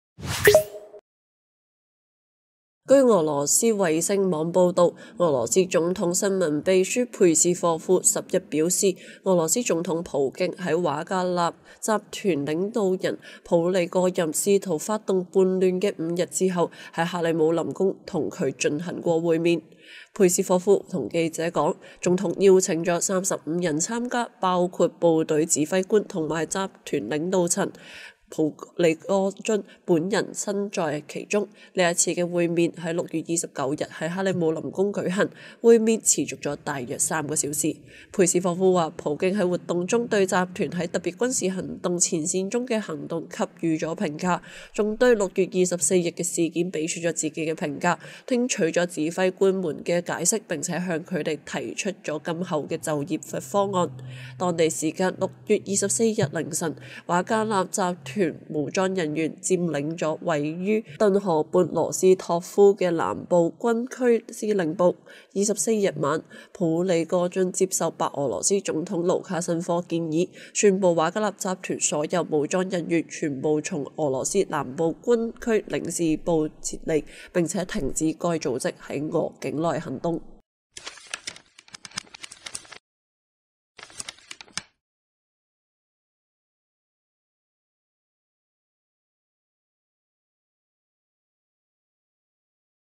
（粤）“叛乱”事发后 普京会见普里戈津
据俄罗斯卫星网报道，俄罗斯总统新闻秘书佩斯科夫10日表示，俄罗斯总统普京在瓦格纳集团领导人普里戈任试图发动叛乱的五天后，在克里姆林宫与其进行过会面。佩斯科夫对记者们说：“总统邀请了35人参加，包括部队指挥官和集团领导层，普里戈津本人身在其中。这次会面于6月29日在克里姆林宫举行。
会面持续了大约三个小时。”